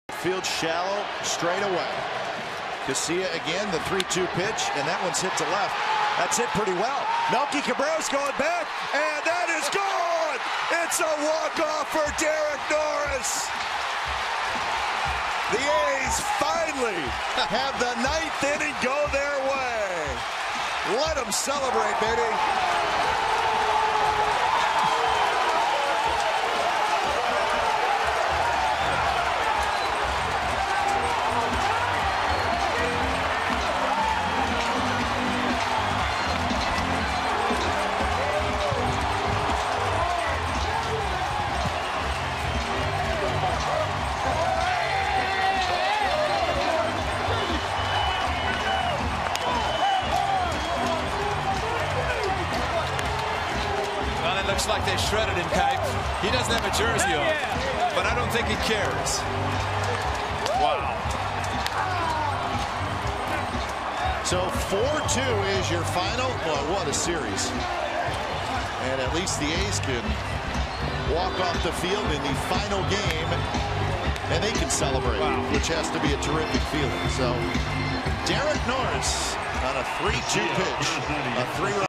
SF@OAK: Norris walks off on dramatic three-run homer
6/24/12: Derek Norris wins the ballgame in the bottom of the ninth with a dramatic, walk-off, three-run homer with two outs